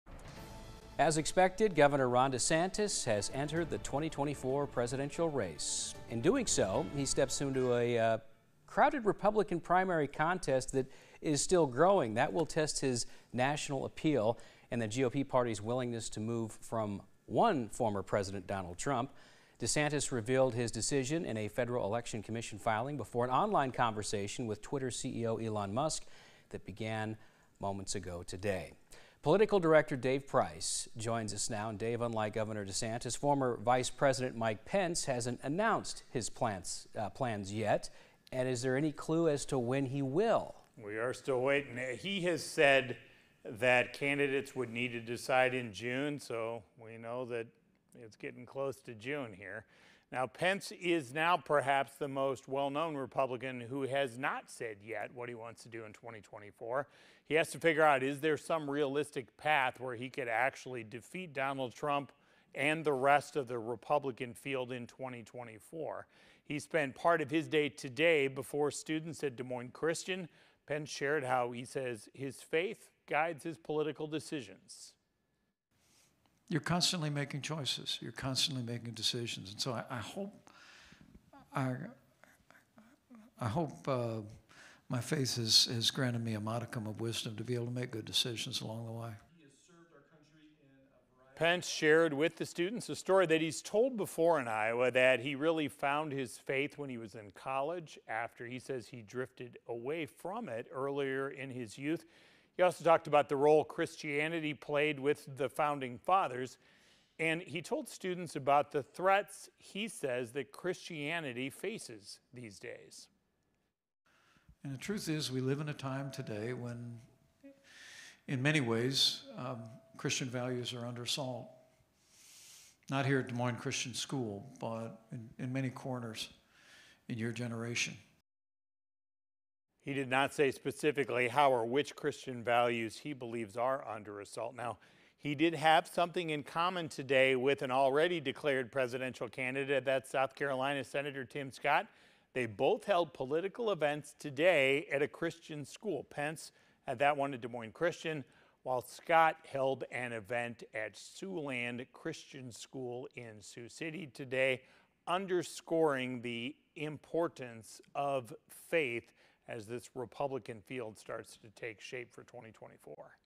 Pence says Christian values "under assault"
Former vice president Mike Pence told a Des Moines audience that Christian values are "under assault"